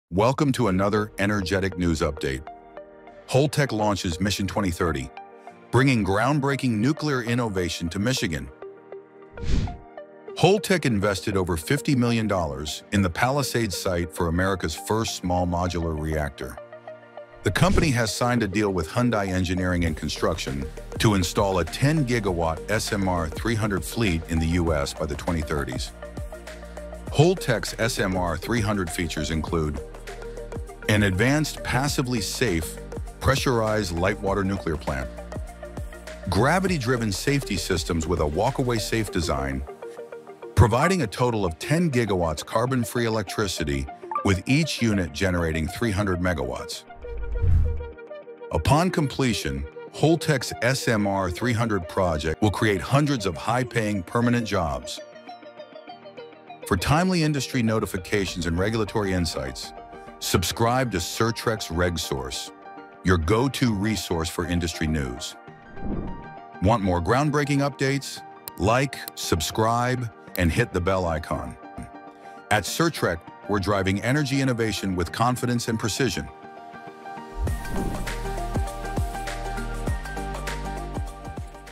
Holtec “Mission 2030”: First SMR-300 Deployment at Palisades, Michigan | Certrec Energetic News
Explore Holtec's groundbreaking $50M investment in Michigan's Palisades site for America's first SMR-300 Nuclear fleet.

This Small Modular Reactor partnership with Hyundai will deliver 10 GW of carbon-free electricity with revolutionary passive safety systems. The project creates hundreds of high-paying nuclear jobs while advancing clean energy technology.

✅ What you Learn:
Details of Holtec's $50 million investment in Palisades site
Features of the innovative SMR-300 technology
How the Hyundai partnership will deploy 10 GW of clean energy
Benefits of gravity-driven safety systems and "walk-away safe" design
Job creation impact of the SMR-300 project in Michigan
How to stay updated with Certrec's RegSource notifications